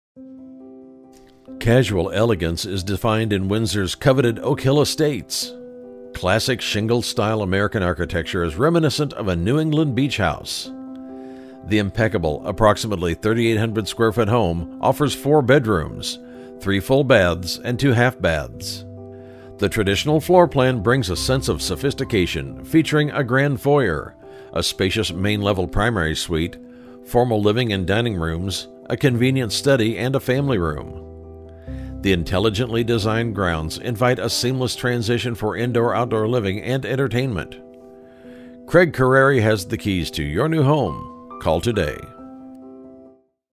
9951 Troon Court Windsor, CA | ColdwellBankerHomes.com
9951 Troon Court, Windsor, CA

Casual elegance is defined in coveted Oak Hill Estates within the shingle style American architecture reminiscent of a New England beach house. The impeccable 3,800+/- square foot home offers four bedrooms, three full bathrooms and two half baths. The spacious main level primary suite provides walk-